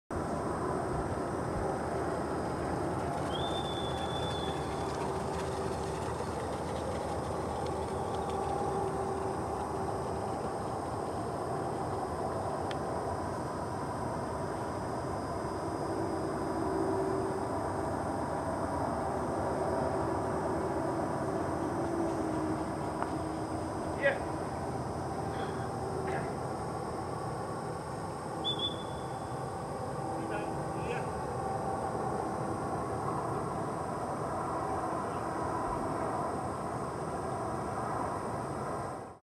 Dog has fun to run - Hund saust durch den Schnee
Funny dog is running through the snow!

Hund hat Spass am Schnee

Filmed with Camcorder Canon HV20